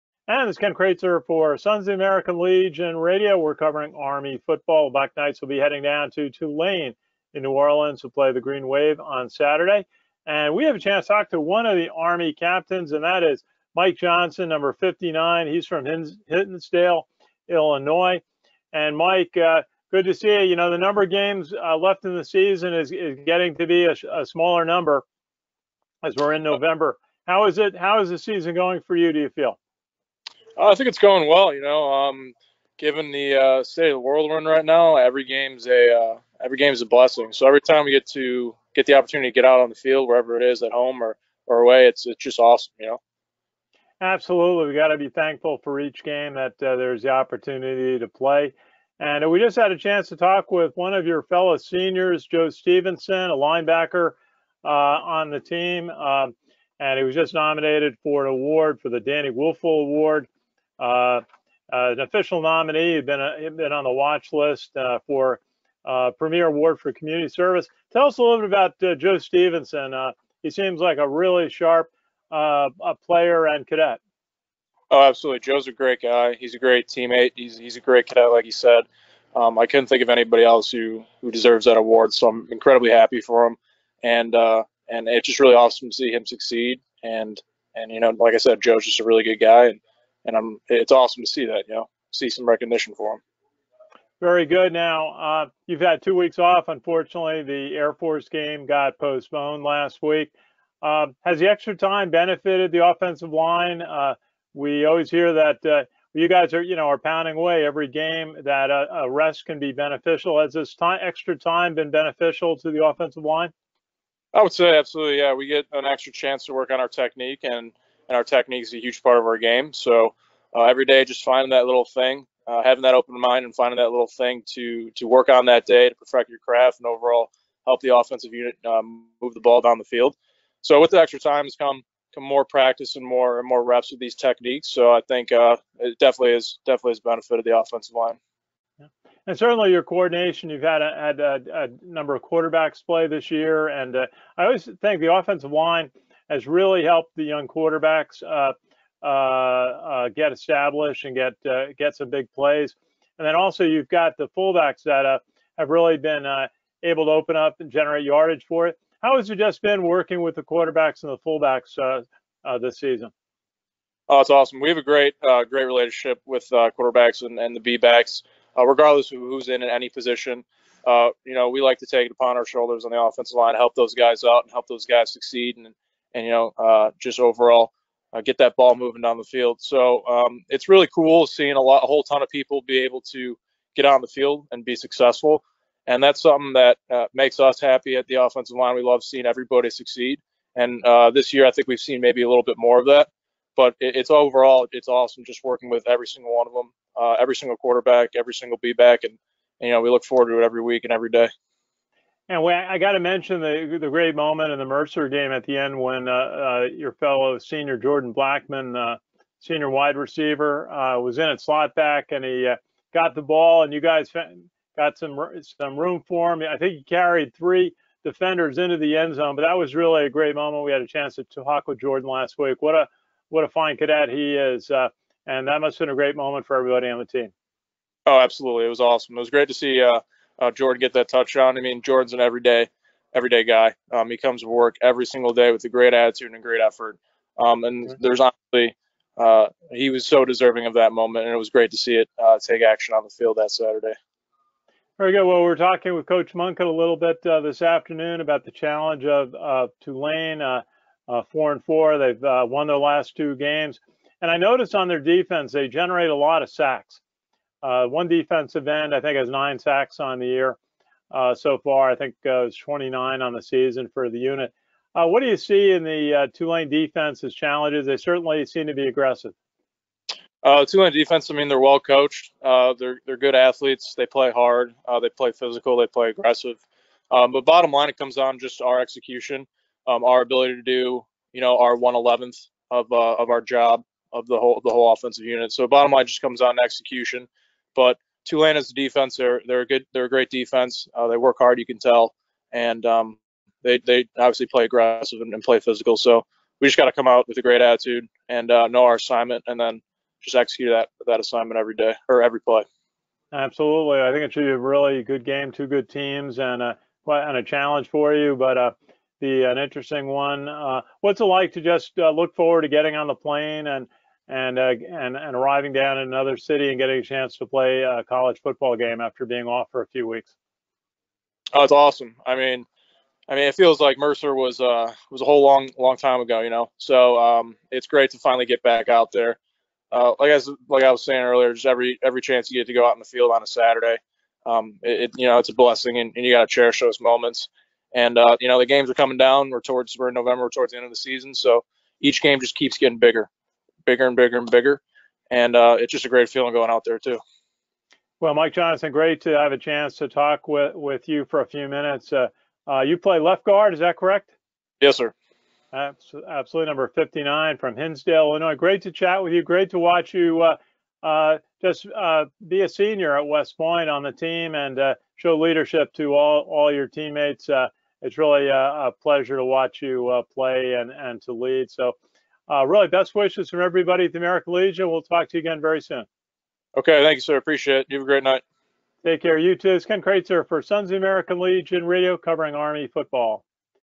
Tulane Week Talk with Army Senior Capt LG Mike Johnson from Hinsdale, IL on SALRadio
SALRadio enjoys a talk with Army West Point senior Left Guard #59 Mike Johnson from Hinsdale, IL.  We cover moving from the postponement of the long awaited Air Force game to prep for Tulane, how the offensive line has worked with the young quarterbacks, approach of "Army Branch Night".
Video by Ken Kraetzer for Sons of the American Legion Radio